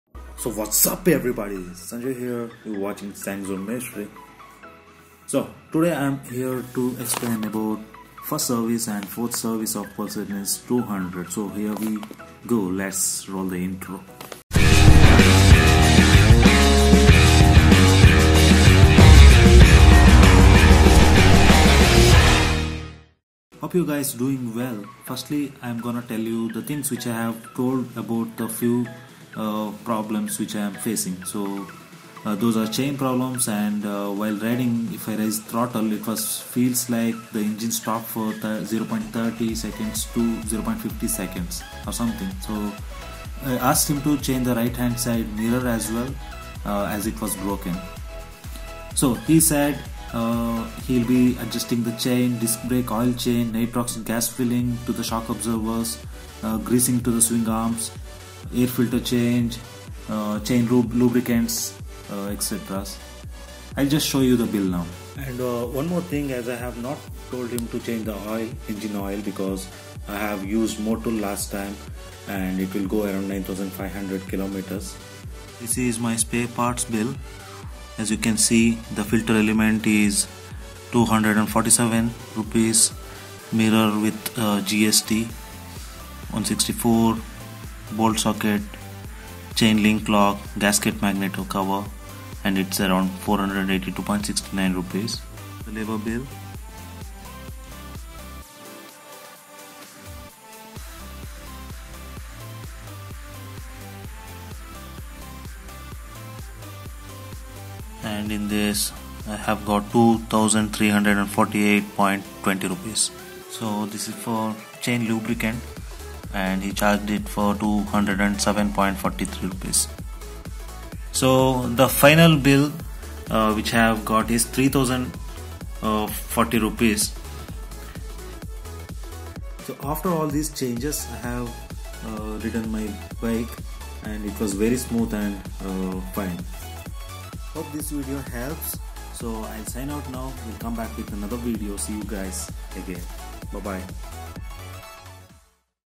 Pulsar NS 200 4th Service - 1st paid service explained with the bill after riding 15000 kms - 2017
Pulsar NS 200 4th Service - 1st paid service explained with the bill